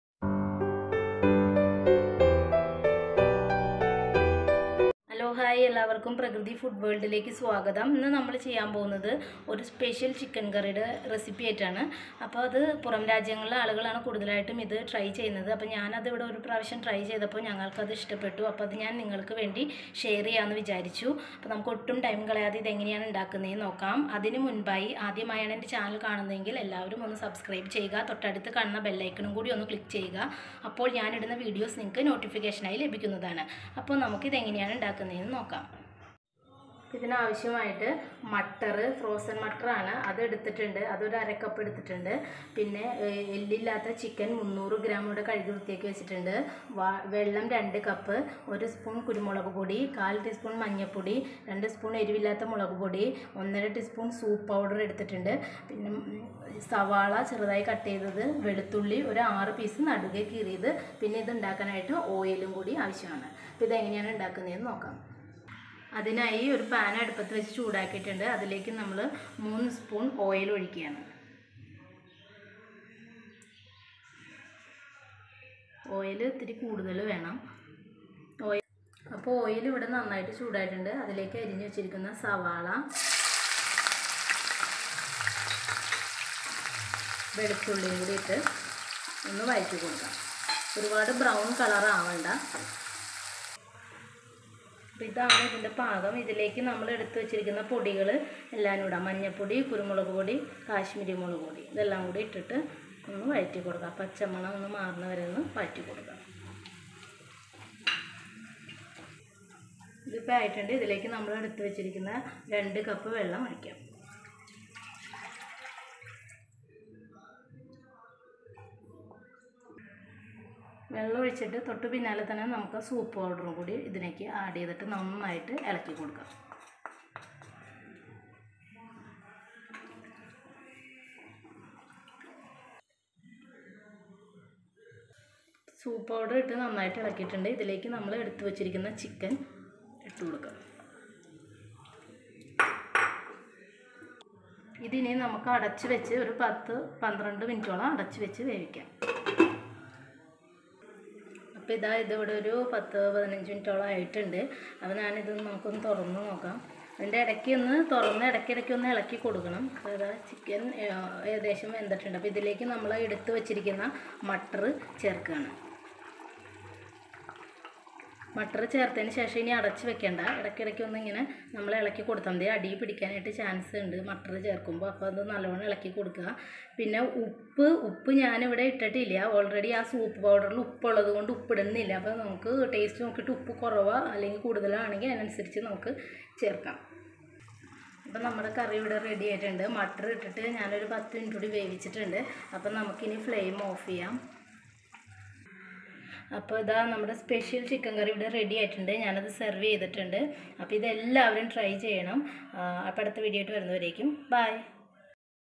Chappathi special chicken curry # ചപ്പാത്തി സ്പെഷ്യൽ ചിക്കൻ കറി
Super testy and simple making chicken curry recipe.
Especially combination with chappathi and porotta

PRAKRITHI FOOD WORLD

# CHAPPATHY SPECIAL CHICKEN CURRY RECIPE

Ingredients :-

Mutter     - half cup
Soup powder  - 1 and half spoon
Majalpodi    -  3 pinch
Kashmeeri mulakupodi - 2 spoon
Oil                - 3 full spoon
Kurumulaku podi   - 1 spoon
Veluthulli     - 6 piece
Savala        - 2 medium size
Vellam        2 cup